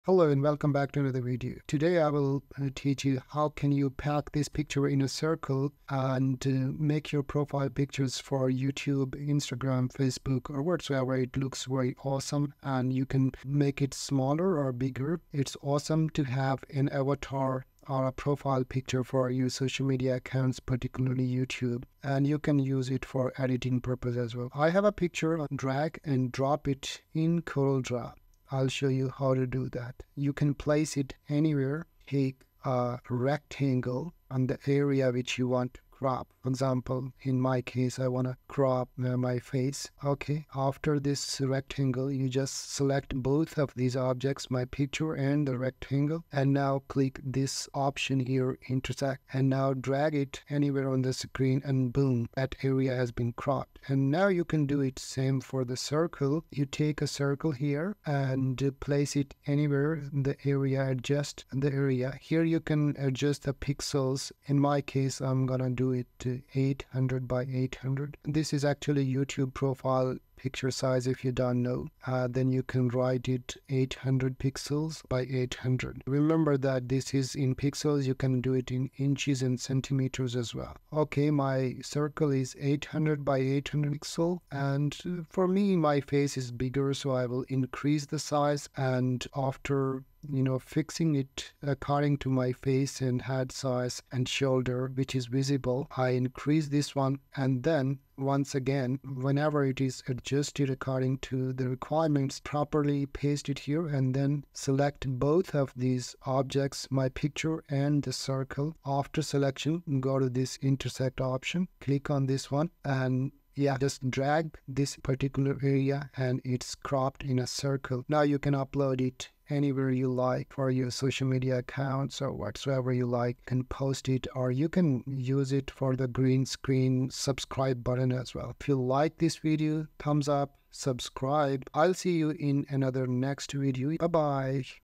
Make Circular Avatars/Profile Photos in CorelDraw (2023)
Welcome to our video on how to crop a photo into a perfect circle or beyond in CorelDraw! (2023 edition)
We will then demonstrate how to crop a photo into a circle in CorelDraw, step by step. This will include how to import your photo into CorelDraw, how to create a circular shape using the Ellipse tool, and how to use the Crop tool to trim the photo to the desired shape.
Throughout the video, we will provide tips and best practices for cropping a photo into a circle in CorelDraw, including how to adjust the size and position of the circle, and how to fine-tune the crop settings.
By the end of this video, you will have all the knowledge and skills you need to crop a photo into a circle in CorelDraw like a pro. So let's get started!